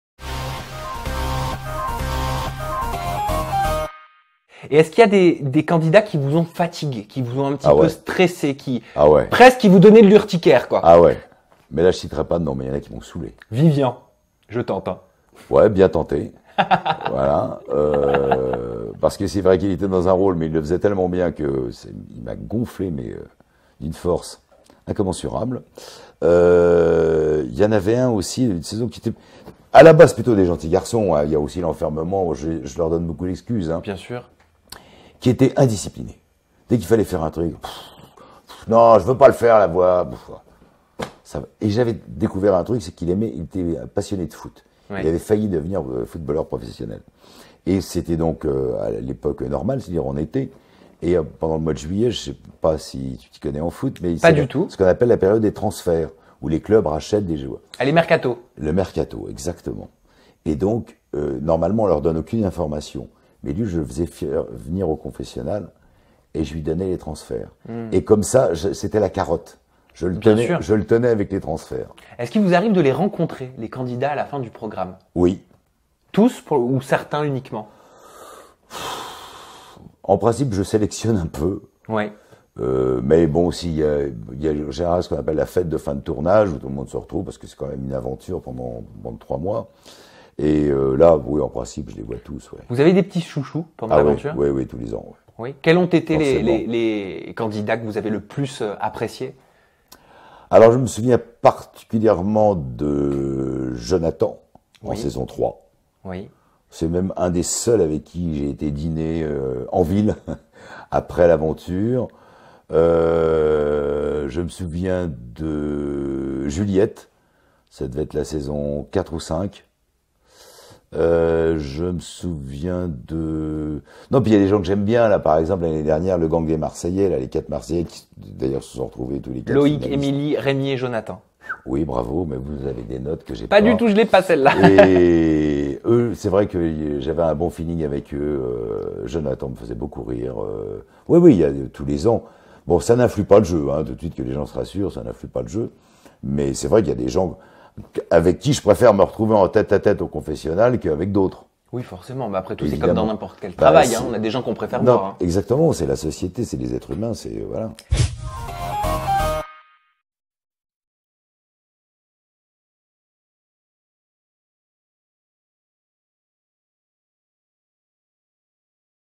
En toute intimité : Dominique Duforest (La voix, Secret Story) : "Jonathan (SS 3) est mon
Dans ce troisième extrait, Sam interviewe Dominique Duforest qui incarne la voix dans Secret Story depuis 2007 !Dans cet extrait, Sam souhaite savoir si certains candidats ont énervé Dominique Duforest durant le tournage. Et sans hésitation, il répond oui !&quot;Vivian (SS 8) était dans un rôle, il le faisait tellement bien… il m’a gonflé d’une force incommensurable ! Il y avait certains garçons qui étaient indisciplinés.&quot;Quand Sam lui demande s'il rencontre les candidats en dehors de l'émission, Dominique sourit :&quot;Oui… en principe, je sélectionne un peu. (…) J’ai des petits chouchous. Jonathan (SS 3) est l’un des seuls avec qui j’ai dîné en ville après l’aventure. Qu’on se rassure, cela n’influe pas sur le jeu !&quot;Découvrez tout de suite le troisième extrait de cet entretien tiré de la nouvelle interview d’En toute intimité de Sam Zirah et réagissez ! Vous pouvez également visionner cette interview dans son intégralité
. Propos recueillis par Sam Zirah Toute reproduction interdite sans la mention Public.